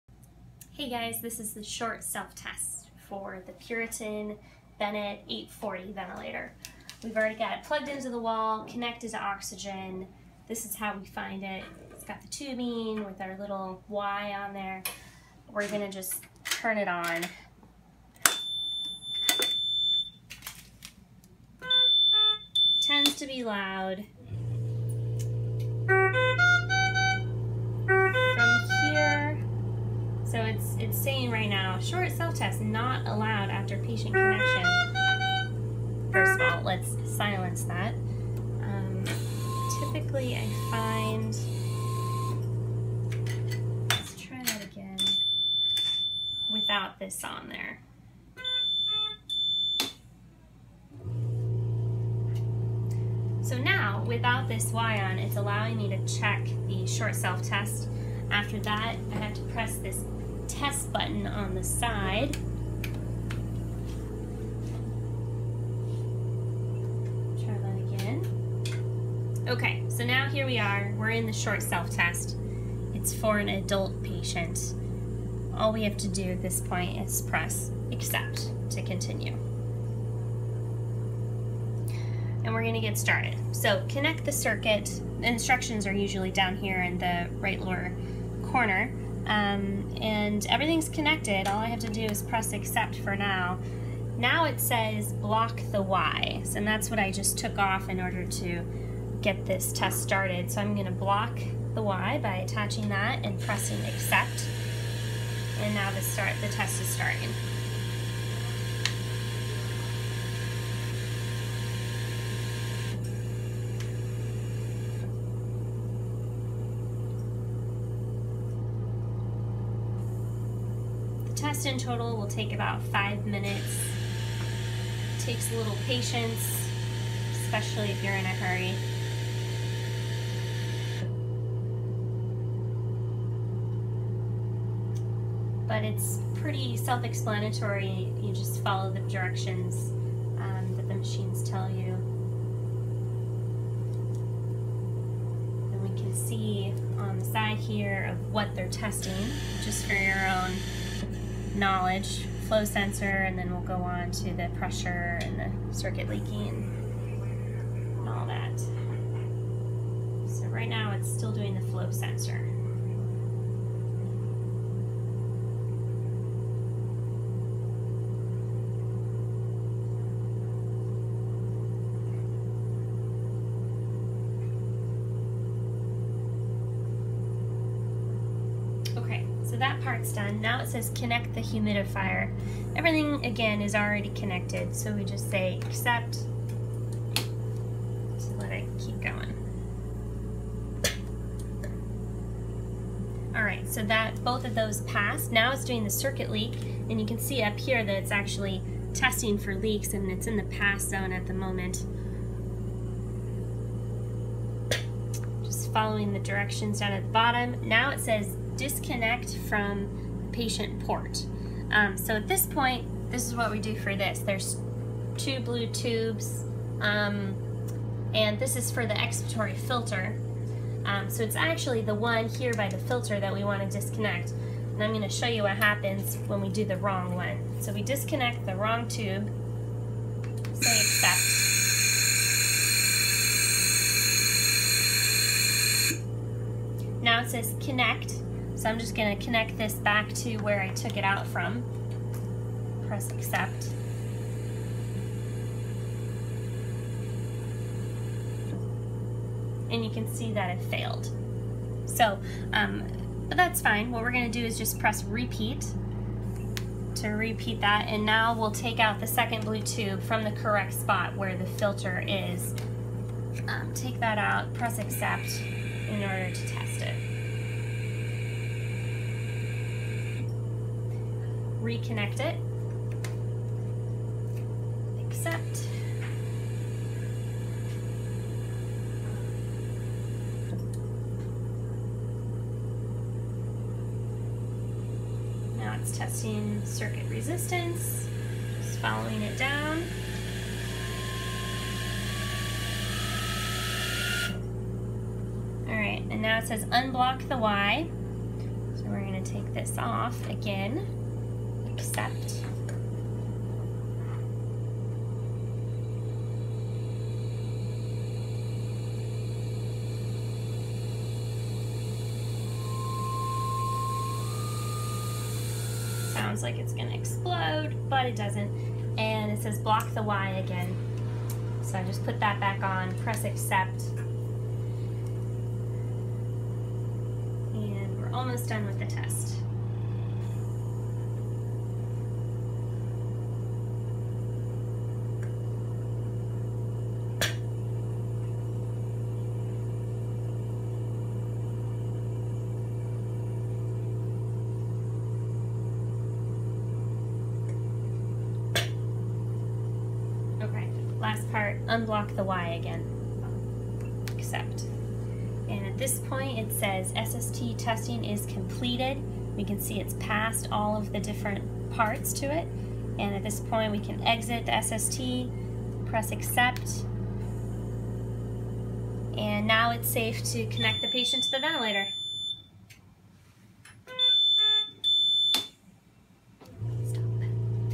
Puritan Bennett 840 Ventilator Short Self Test SST
How to do a ventilator short self test (SST) to make sure everything is working prior to use.